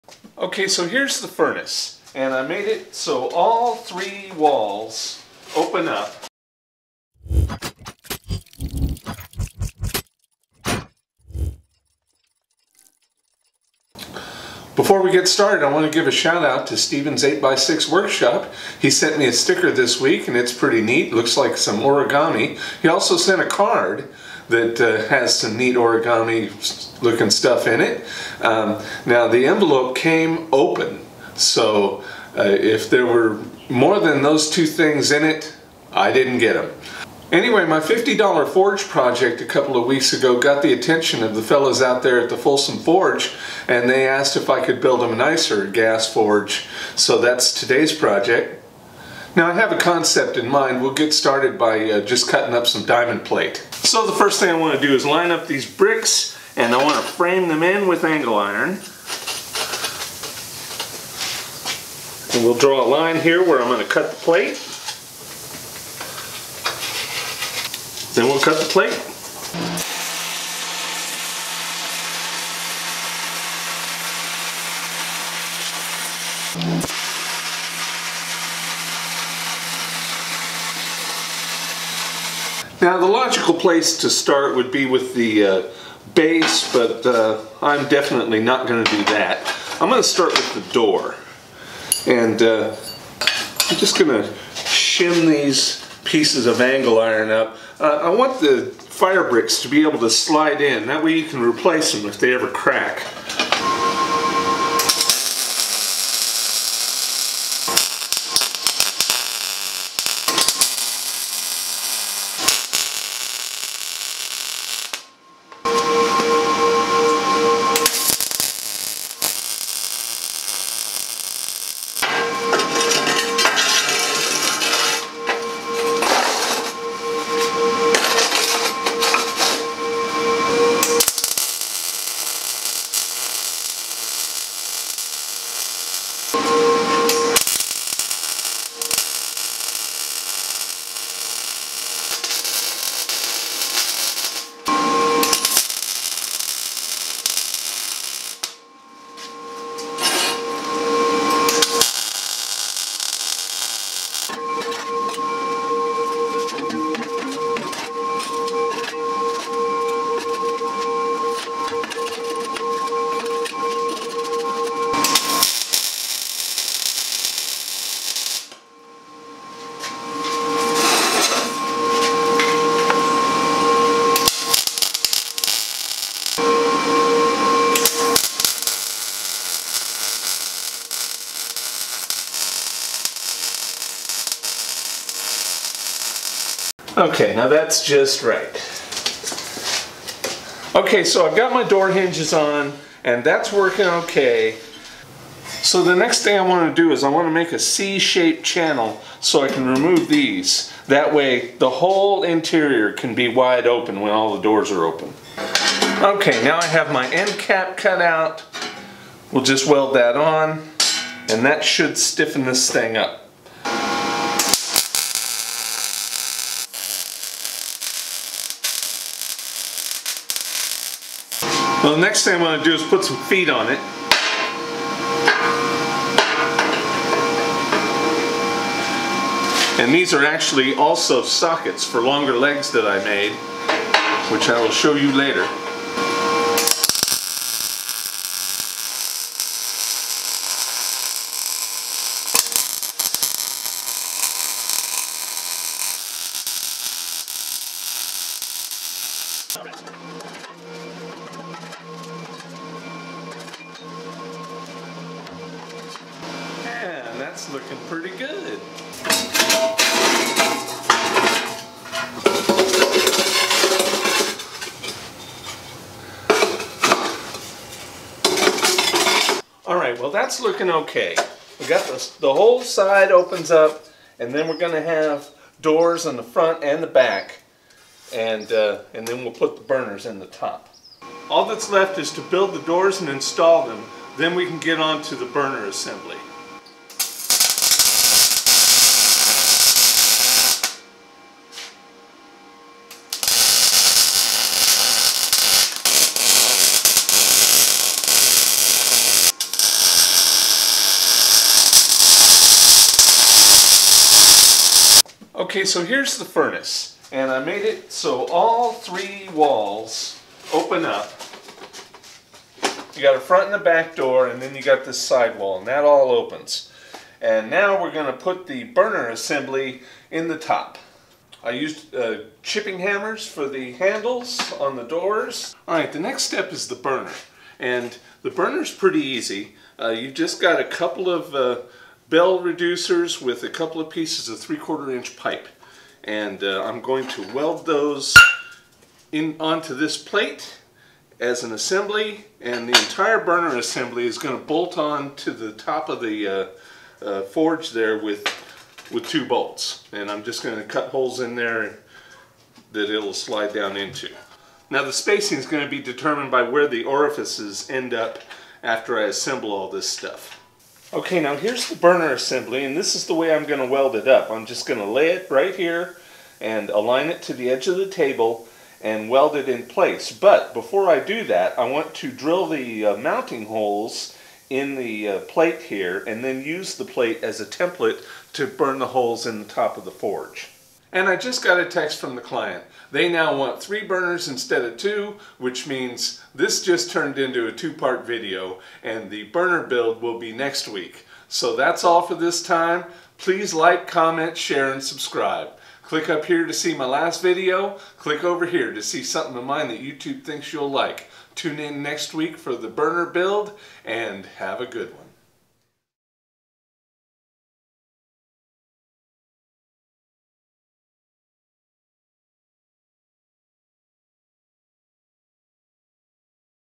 Propane Forge Redux! Part 1 - The Furnace
The folks at the Folsom Forge asked me to make a really nice propane forge for them. This week we begin with the furnace. Next week, we'll focus on the burner assembly.

These are places where you can see more of my work.

Get a free sticker by either sending one of your stickers or an S.A.S.E. to...